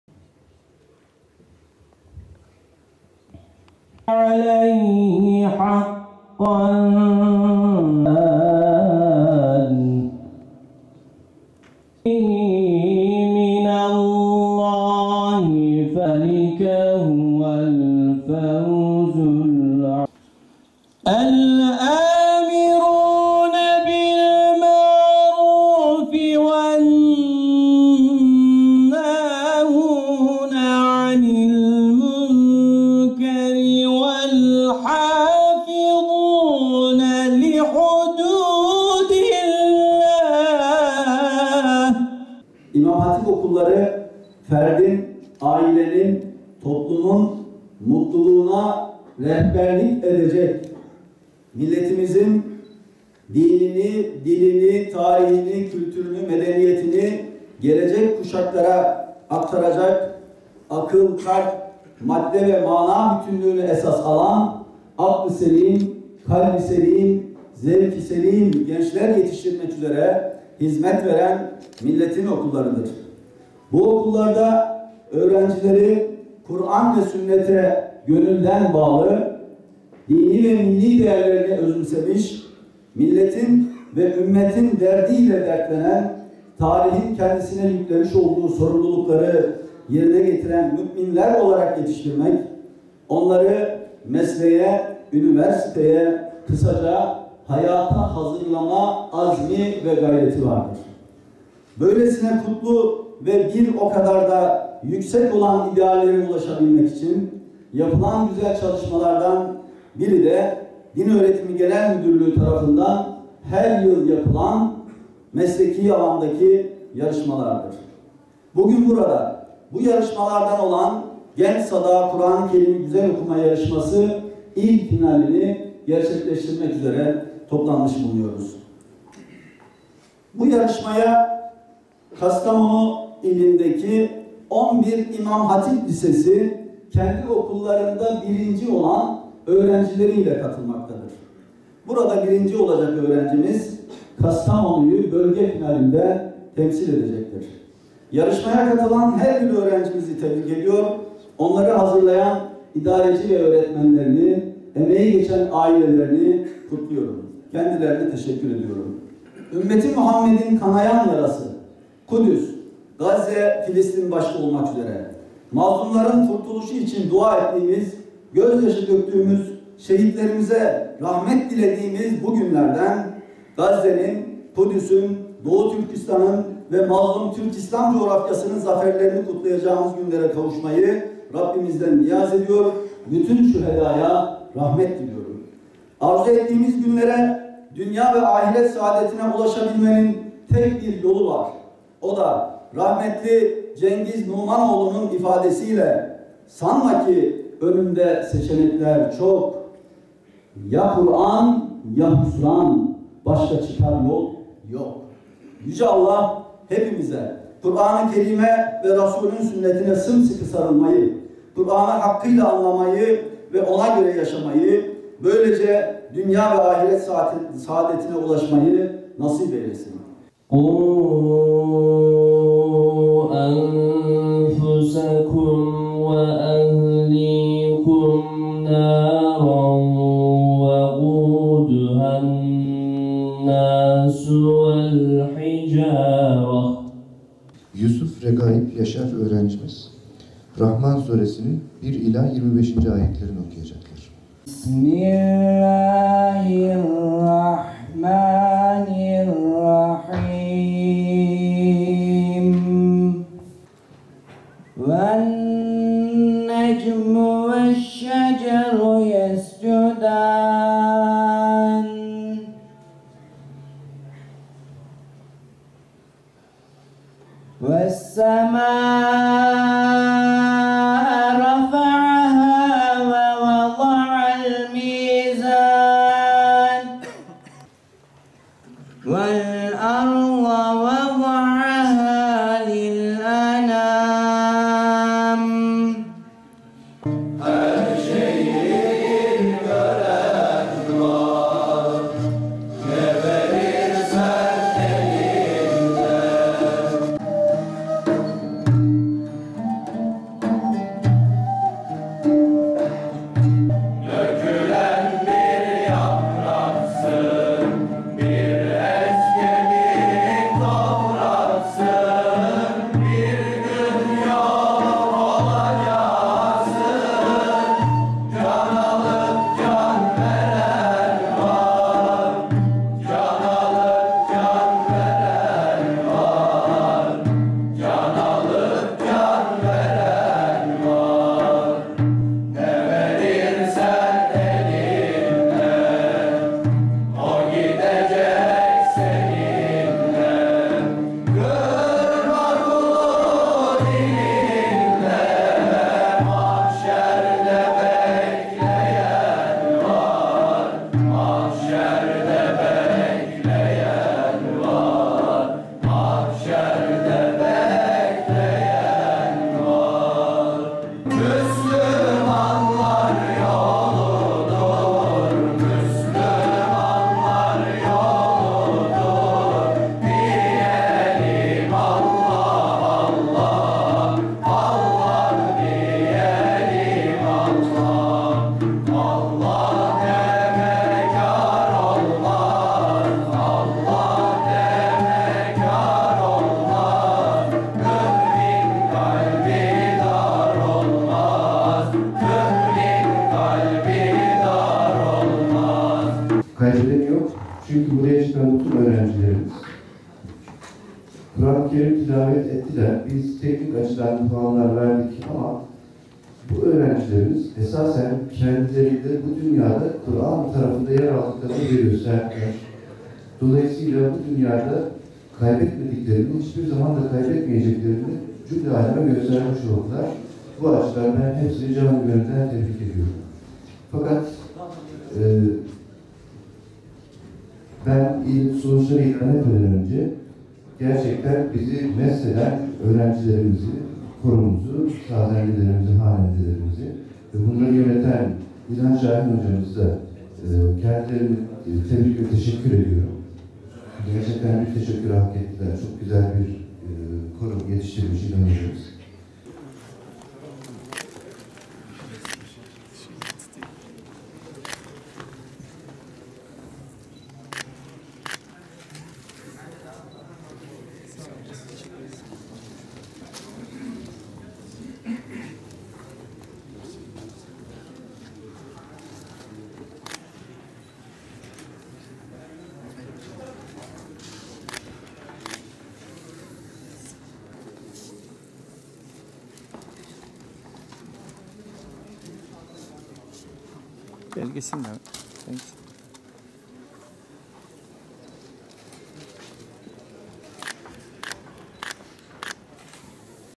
Kur’an-ı Kerim’i en güzel şekilde okumak için yarıştılar
Kastamonu’da imam hatip lisesi öğrencileri arasında düzenlenen “Genç Sada Kur’an-ı Kerim’i Güzel Okuma Yarışması’nda öğrenciler, Kur’an-ı Kerim’i en güzel şekilde okumak için yarıştı.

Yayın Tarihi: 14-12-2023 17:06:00